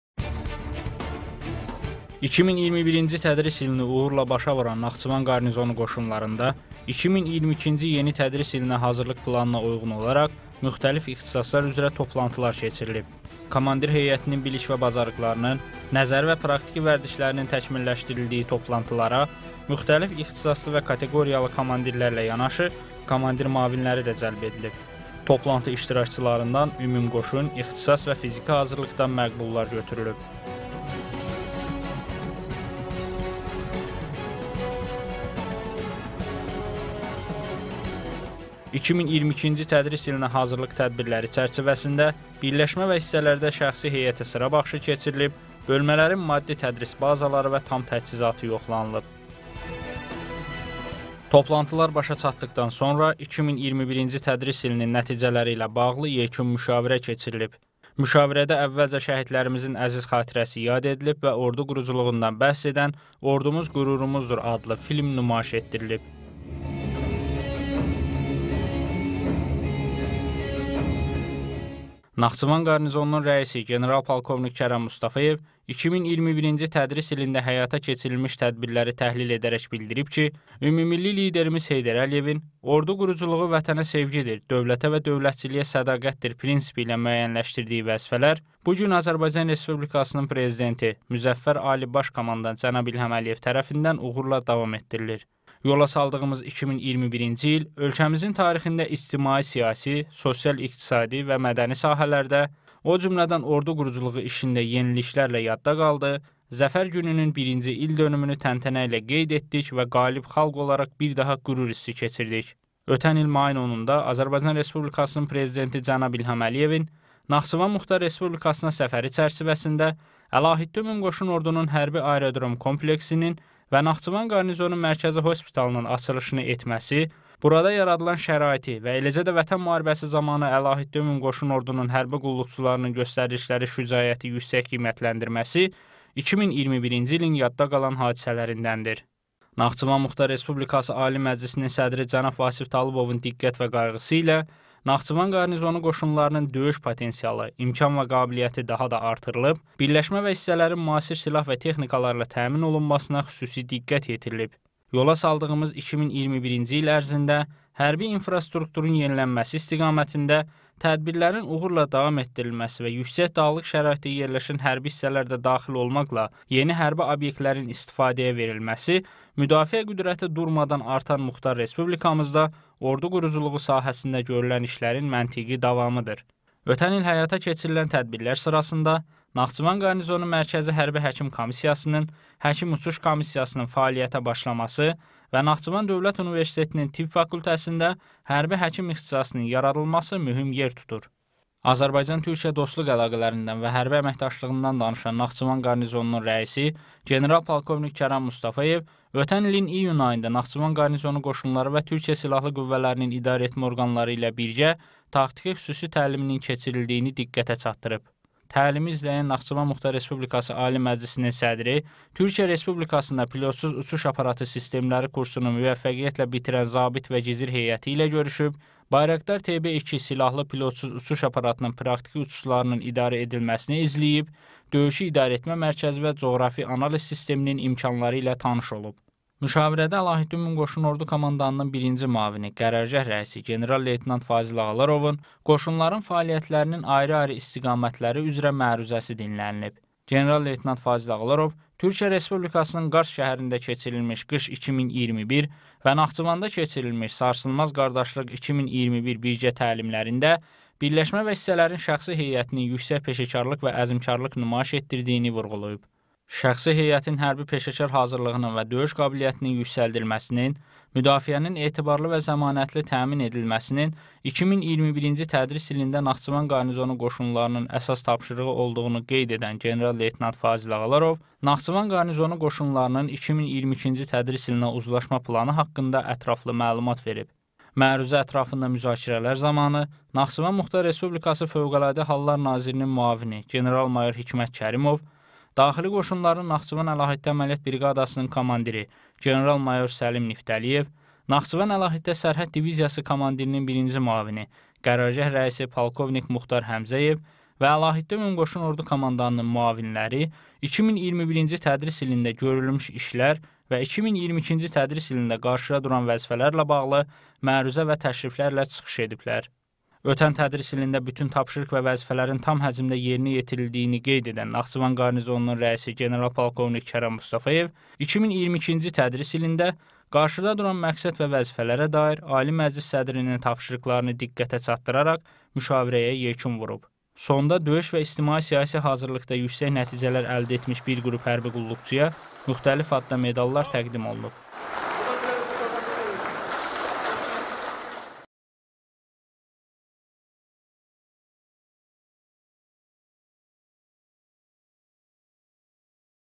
Naxçıvan Qarnizonu Qoşunlarında müşavirə keçirilib
2021-ci tədris ilini uğurla başa vuran Naxçıvan Qarnizonu Qoşunlarında 2022-ci yeni tədris ilinə hazırlıq planına uyğun olaraq müxtəlif ixtisaslar üzrə toplantılar keçirilib. Komandir heyətinin bilik və bacarıqlarının, nəzəri və praktiki vərdişlərinin təkmilləşdirildiyi toplantılara müxtəlif ixtisaslı və kateqoriyalı komandirlərlə yanaşı komandir müavinləri də cəlb edilib.

Toplantı iştirakçılarından ümumqoşun, ixtisas və fiziki hazırlıqdan məqbullar götürülüb. 2022-ci tədris ilinə hazırlıq tədbirləri çərçivəsində birləşmə və hissələrdə şəxsi heyətə sıra baxışı keçirilib, bölmələrin maddi-tədris bazaları və tam təchizatı yoxlanılıb.

Toplantılar başa çatdıqdan sonra 2021-ci tədris ilinin nəticələri ilə bağlı yekun müşavirə keçirilib. Müşavirədə əvvəlcə şəhidlərimizin əziz xatirəsi yad edilib və ordu quruculuğundan bəhs edən “Ordumuz qürurumuzdur” adlı film nümayiş etdirilib.

Naxçıvan Qarnizonunun rəisi, general-polkovnik Kərəm Mustafayev 2021-ci tədris ilində həyata keçirilmiş tədbirləri təhlil edərək bildirib ki, ümummilli liderimiz Heydər Əliyevin “Ordu quruculuğu Vətənə sevgidir, dövlətə və dövlətçiliyə sədaqətdir” prinsipi ilə müəyyənləşdirdiyi vəzifələr bu gün Azərbaycan Respublikasının Prezidenti, Müzəffər Ali Baş Komandan cənab İlham Əliyev tərəfindən uğurla davam etdirilir. Yola saldığımız 2021-ci il ölkəmizin tarixində ictimai-siyasi, sosial-iqtisadi və mədəni sahələrdə, o cümlədən Ordu quruculuğu işində yeniliklərlə yadda qaldı, “Zəfər Günü”nün birinci ildönümünü təntənə ilə qeyd etdik və qalib xalq olaraq bir daha qürur hissi keçirdik. Ötən il mayın 10-da Azərbaycan Respublikasının Prezidenti cənab İlham Əliyevin Naxçıvan Muxtar Respublikasına səfəri çərçivəsində Əlahiddə Ümumqoşun Ordunun hərbi aerodrom kompleksinin və Naxçıvan Qarnizonu Mərkəzi Hospitalının açılışını etməsi, burada yaradılan şəraiti və eləcə də Vətən Müharibəsi zamanı Əlahiddə Ümumqoşun Ordunun hərbi qulluqçularının göstərdikləri şücaəti yüksək qiymətləndirməsi 2021-ci ilin yadda qalan hadisələrindəndir.

Naxçıvan Muxtar Respublikası Ali Məclisinin Sədri cənab Vasif Talıbovun diqqət və qayğısı ilə Naxçıvan Qarnizonu Qoşunlarının döyüş potensialı, imkan və qabiliyyəti daha da artırılıb, birləşmə və hissələrin müasir silah və texnikalarla təmin olunmasına xüsusi diqqət yetirilib. Yola saldığımız 2021-ci il ərzində hərbi infrastrukturun yenilənməsi istiqamətində tədbirlərin uğurla davam etdirilməsi və yüksək dağlıq şəraitdə yerləşən hərbi hissələr də daxil olmaqla yeni hərbi obyektlərin istifadəyə verilməsi müdafiə qüdrəti durmadan artan muxtar respublikamızda ordu quruculuğu sahəsində görülən işlərin məntiqi davamıdır. Ötən il həyata keçirilən tədbirlər sırasında Naxçıvan Qarnizonu Mərkəzi Hərbi-Həkim Komissiyasının (həkim-uçuş komissiyasının) fəaliyyətə başlaması və Naxçıvan Dövlət Universitetinin Tibb Fakültəsində “Hərbi həkim” ixtisasının yaradılması mühüm yer tutur.